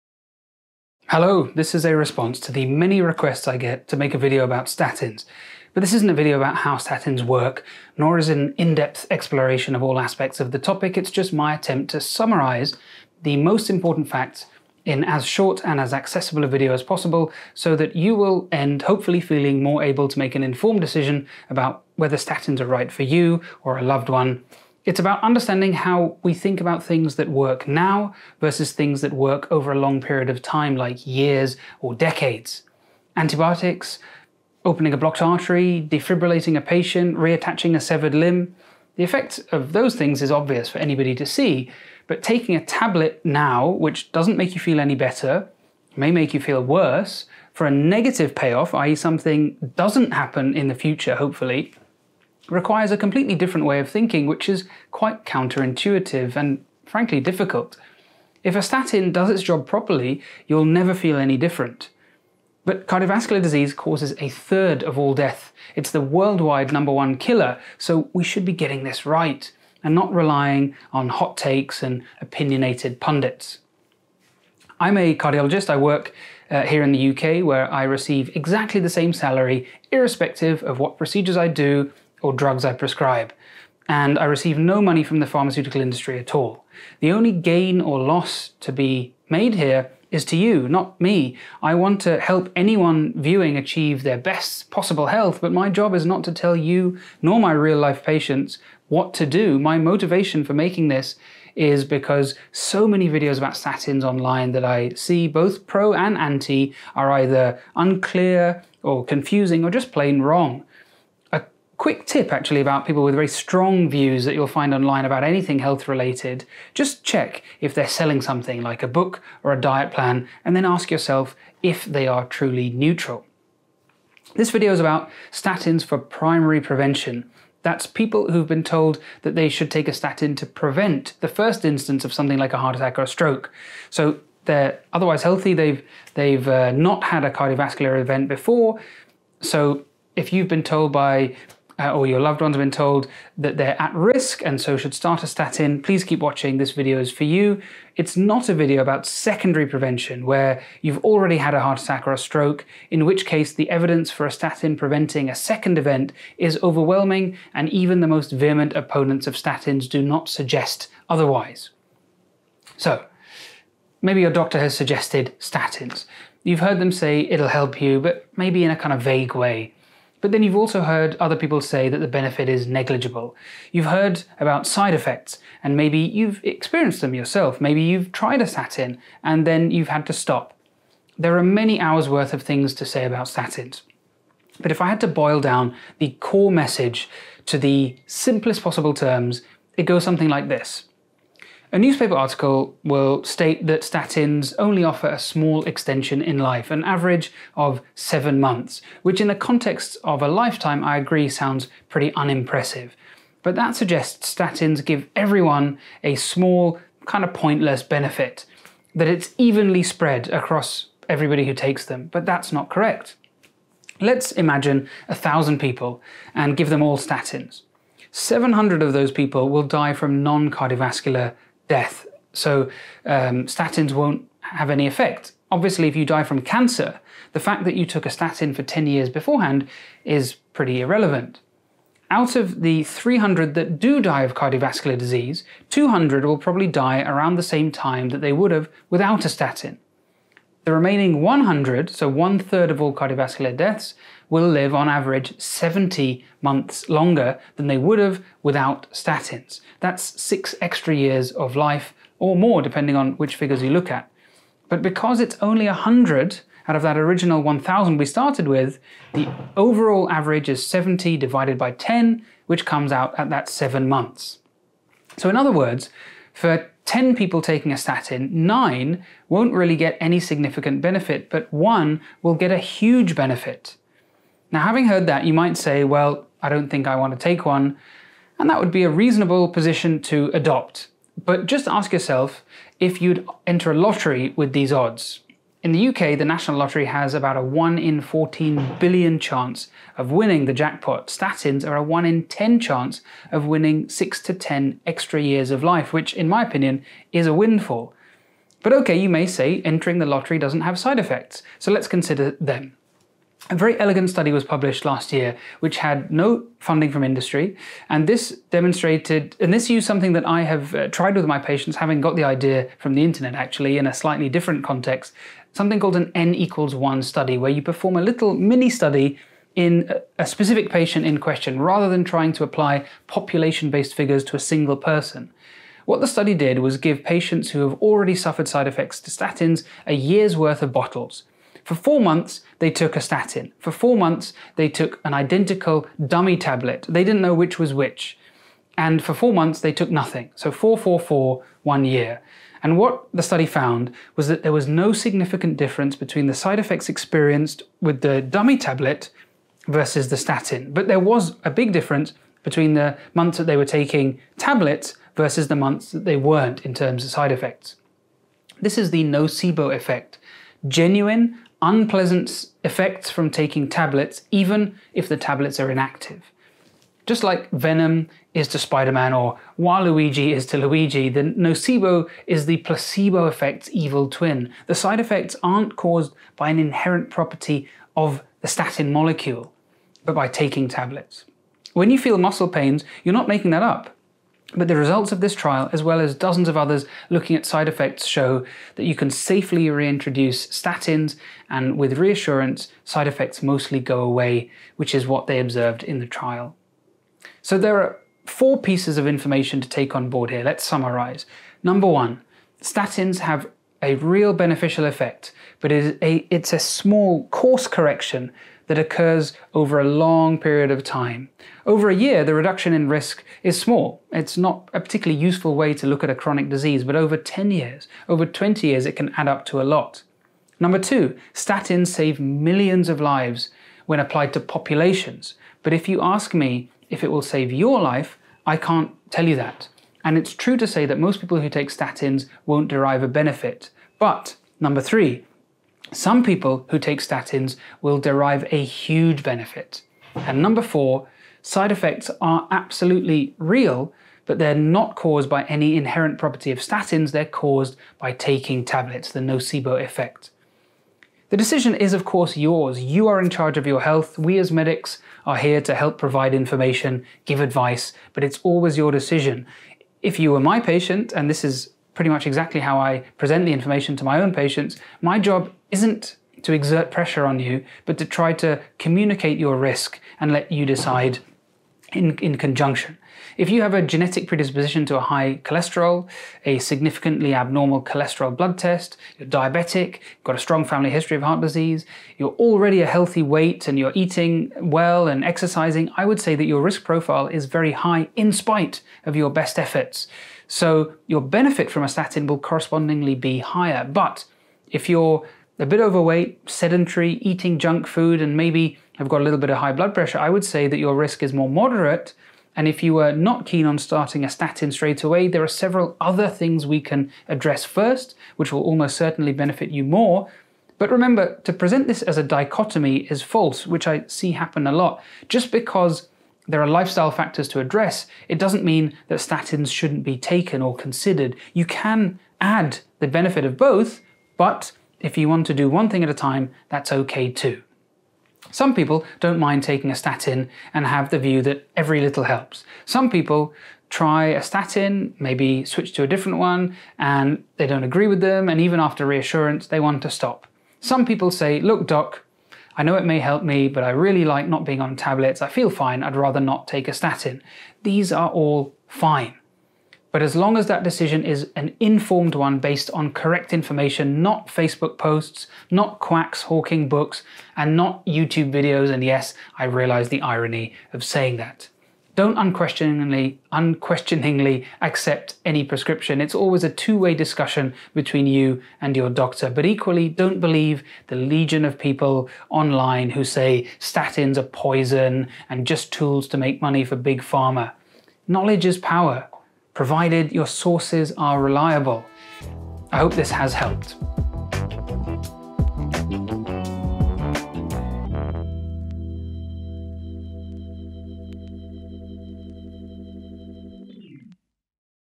Statins Explained in 10 Minutes (by a cardiologist)
I've tried to boil down one of the most complicated topics in medicine to a YouTube length. Let's hope it worked! Statins are the most prescribed medication in the world but most of what I read and watch online is either completely vague so as to be useless, or just plain wrong.

This is one of the most requested topics I get, and while not the most exciting, it's an important one. So I hope it helps at least one person make a decision.

I wanted to keep it short to ensure people actually watched - an in-depth lecture would of course have offered the chance to really dive into the details but my objective here is to help as many people as possible.

If you find statins confusing, you're not alone! Cardiologists and GPs probably prescribe these more often than anyone else and yet even I took a long time to really understand the enormous mass of data and coverage (which of course is impossible to read in its entirety). I want to specifically credit Professor Darrel Francis from Imperial College in London for helping me see statins in the kind of reference frame that I present in the video. Prior to that, my understanding was that kind of vague "oh this will help you, probably. I think." that you might get if you just read guidelines, which of course exist to maximise health for a whole population.